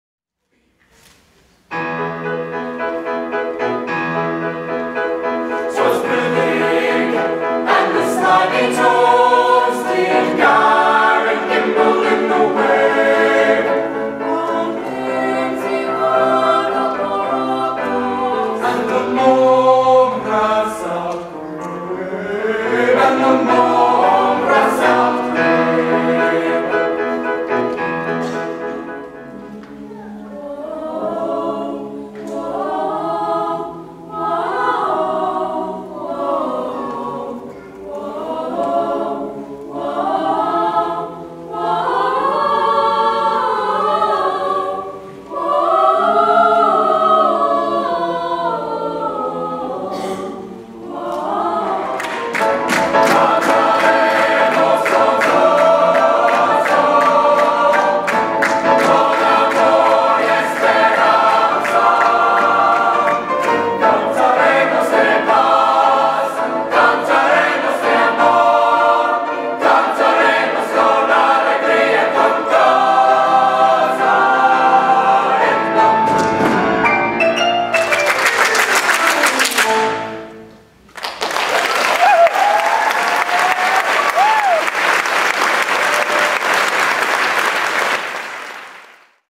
FSK 2013 Spring Chorus Concert
This is a brief recap of the 2013 Spring Chorus Concert at Francis Scott Key High School. Featured here are the Chamber Singers followed by the Varsity Chorale.